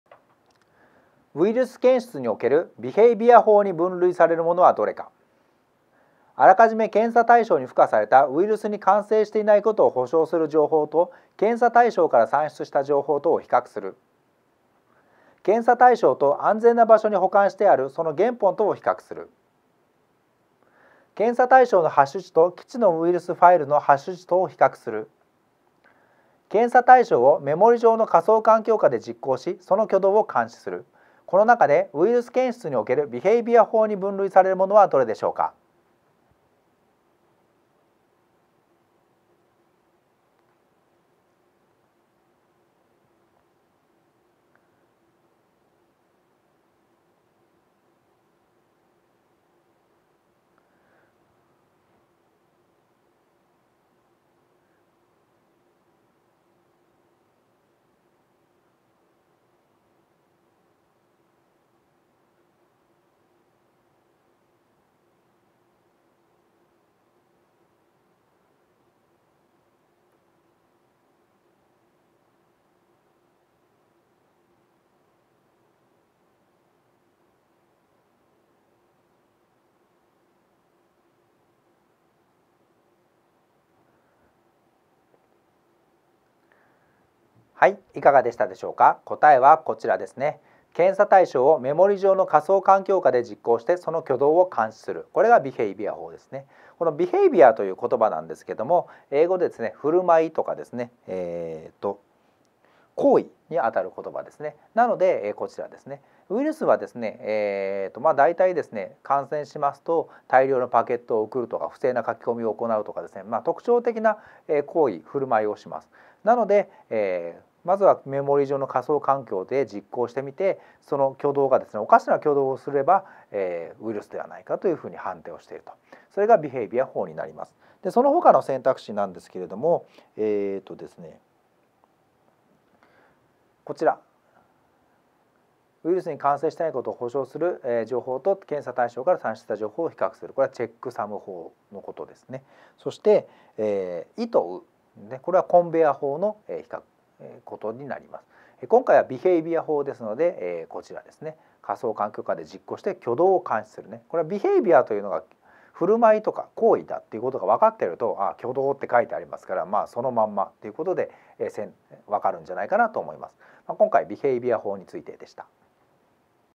問18 ウイルス検出におけるビヘイビア法に分類されるものはどれか。
情報セキュリティマネジメント試験　過去問演習

ア あらかじめ検査対象に付加された，ウイルスに感染していないことを保証する情報と，検査対象から算出した情報とを比較する。

イ 検査対象と安全な場所に保管してあるその原本とを比較する。

ウ 検査対象の ハッシュ値と既知のウイルスファイルのハッシュ値とを比較する。

エ 検査対象をメモリ上の仮想環境下で実行して，その挙動を監視する。